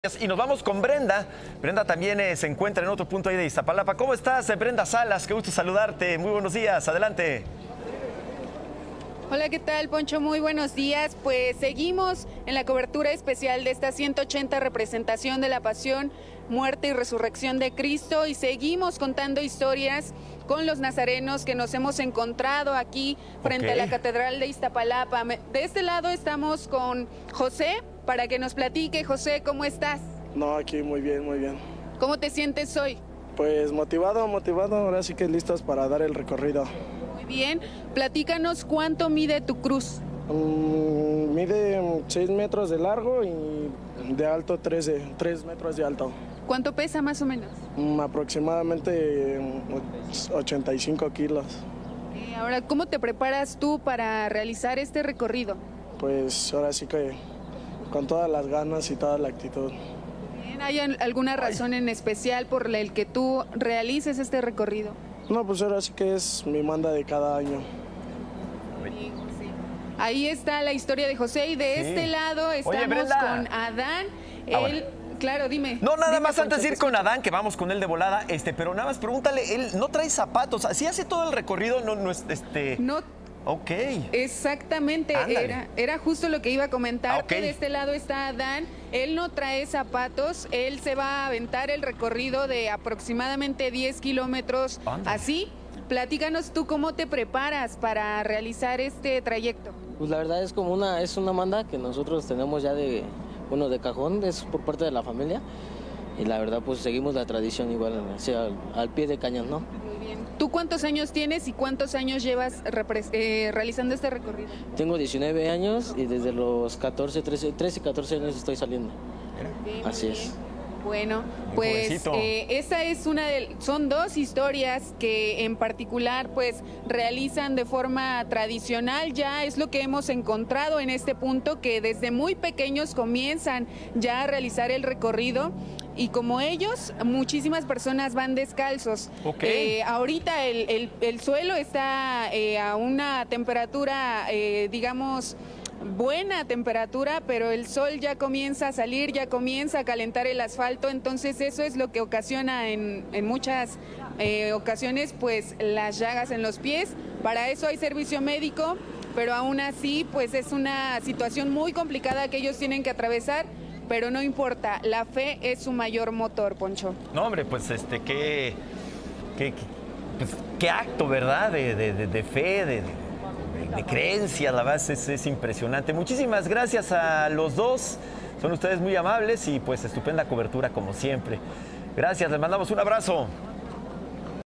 ¿Cómo se preparan los nazarenos para la procesión? | De Pisa y Corre
Algunos de los nazarenos nos cuentan la historia de cómo se prepararon para realizar el recorrido durante la procesión de SemanaSanta.